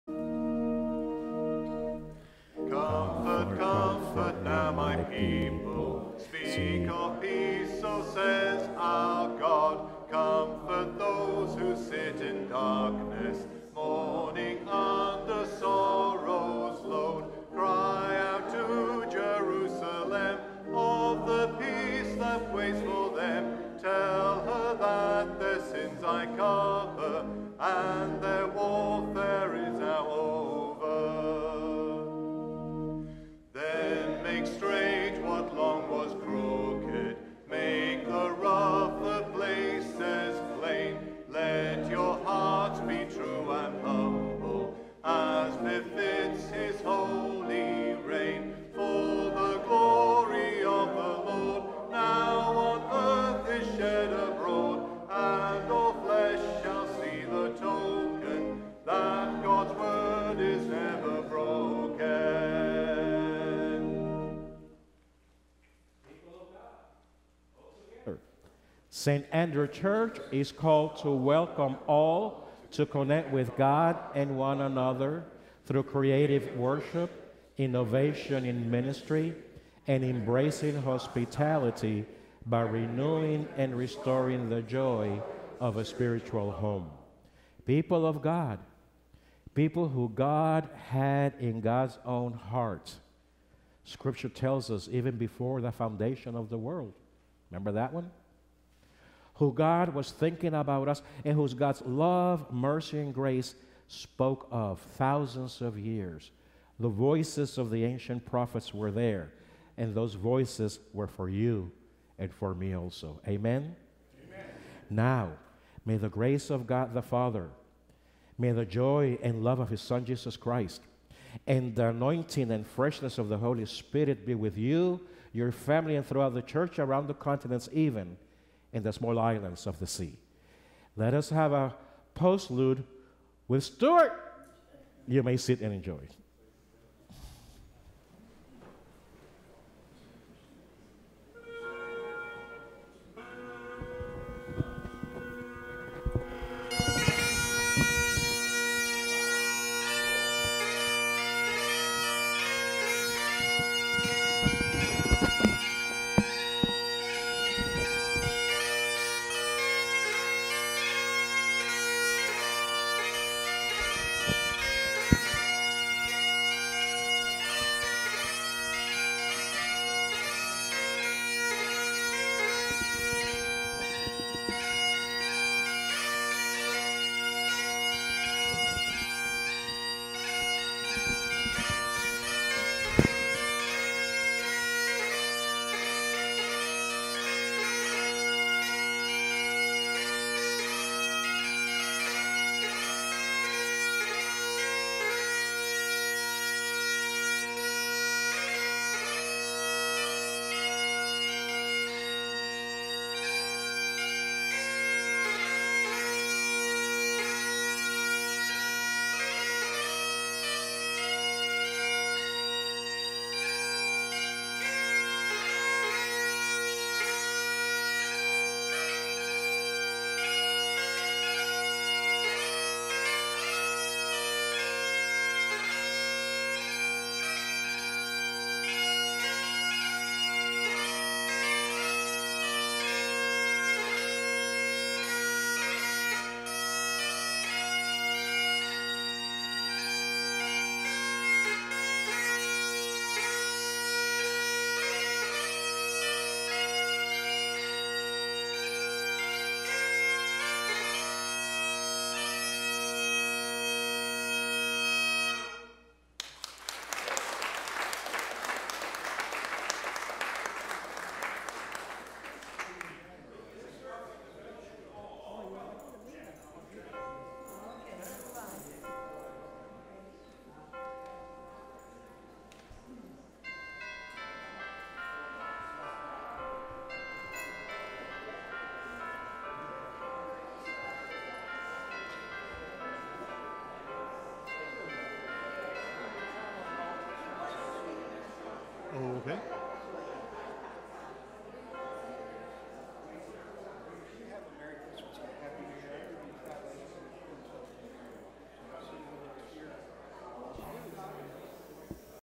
St Andrew PB Church, SCC FL - November 27, 2022 part 2
November 27th, 2022 Service - Who Was There? Advent Series and St Andrews Day - from St Andrew Presbyterian Church, Sun City Center Florida part 2